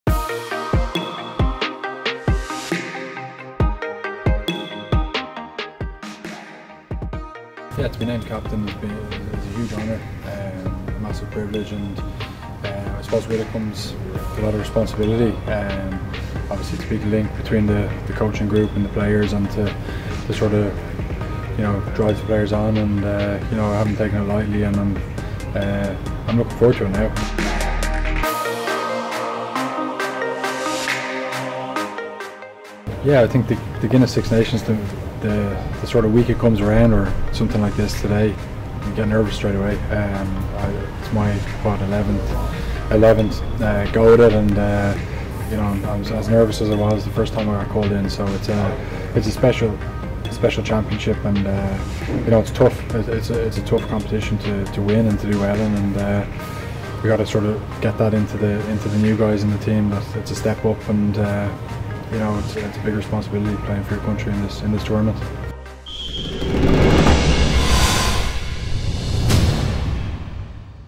Captain's Preview: Johnny Sexton | 2020 Guinness Six Nations
Ireland captain Johnny Sexton previews the 2020 Guinness Six Nations.